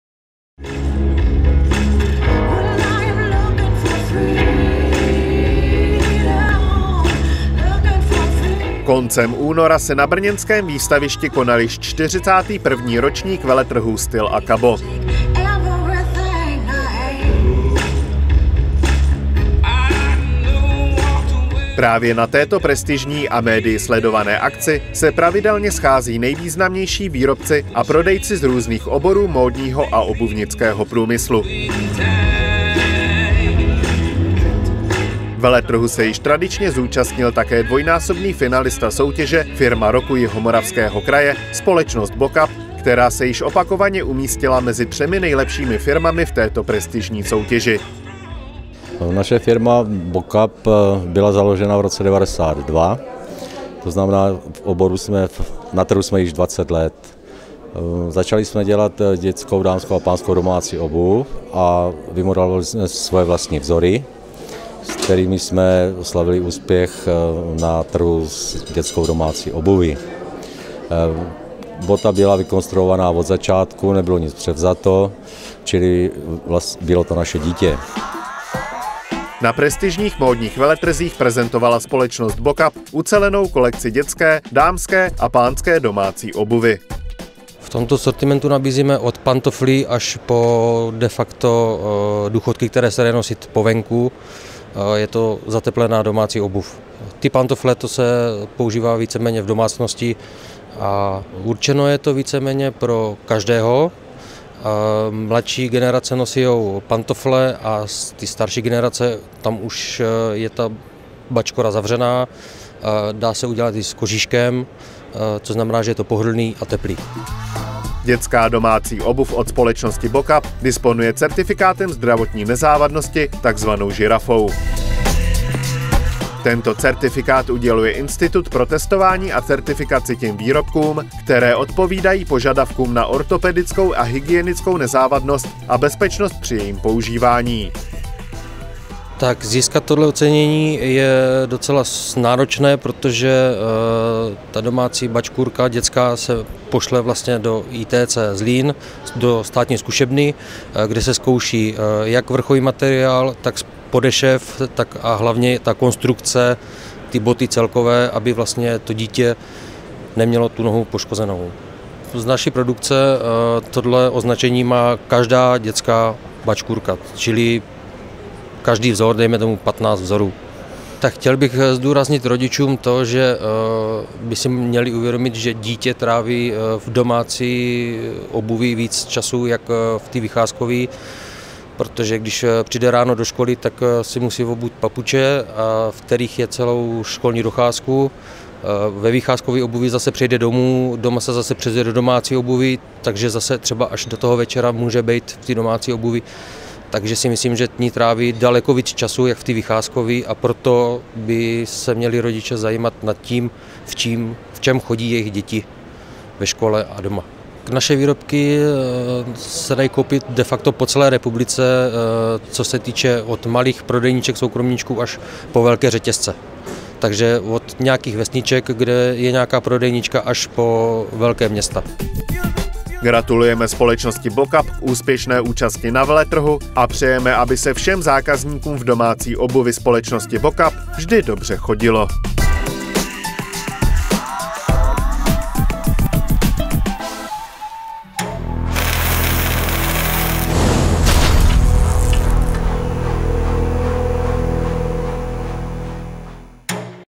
BOKAP s.r.o. na veletrhu Styl a Kabo 2013
Veletrhu se Styl a Kabo 2013 již tradičně zúčastnil také dvojnásobný finalista soutěže Firma roku Jihomoravského kraje - společnost Bokap, která se již opakovaně umístila mezi třemi nejlepšími firmami v této prestižní soutěži. Na prestižních módních veletrzích prezentovala společnost Bokap ucelenou kolekci dětské, dámské a pánské domácí obuvi.
Dětská domácí obuv od společnosti Bokap disponuje certifikátem zdravotní nezávadnosti -- takzvanou Žirafou. Tento certifikát uděluje Institut pro testování a certifikaci těm výrobkům, které odpovídají požadavkům na ortopedickou a hygienickou nezávadnost a bezpečnost při jejím používání.